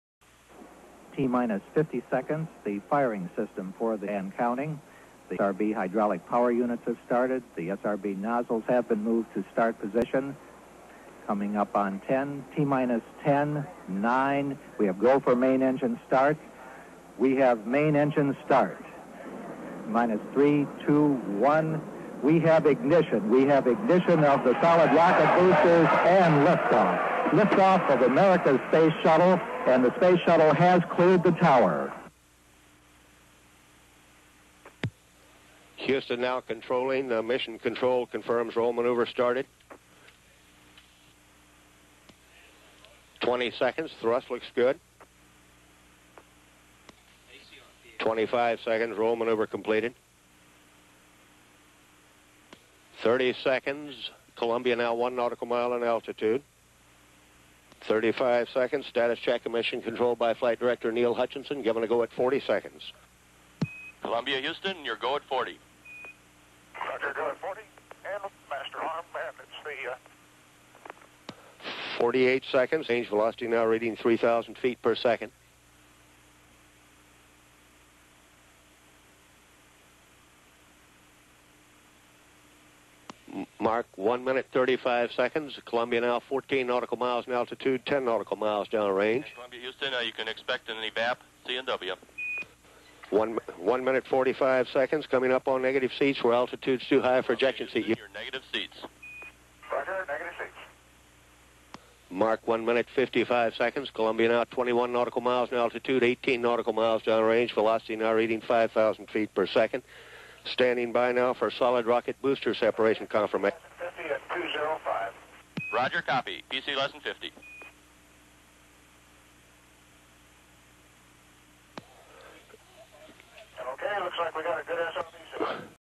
STS-2 Space Shuttle Columbia Launch
STS - 2 is the secound Mission of the Orbiter Columbia